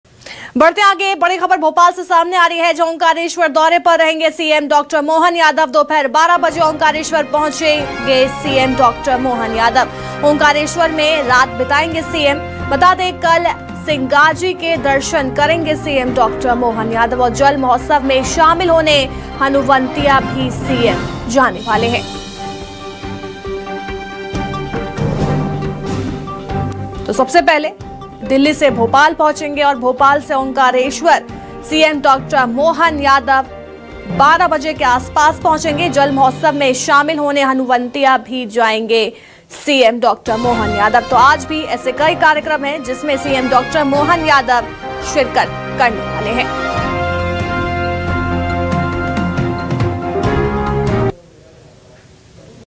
Madhya Pradesh : ओंकारेश्वर दौरे पर रहेंगे सीएम मोहन यादव, कल करेंगे सिंगाजी के दर्शन | News24 MPCG
Madhya Pradesh News : ओंकारेश्वर दौरे पर रहेंगे सीएम मोहन यादव, कल करेंगे सिंगाजी के दर्शन | News24 MPCG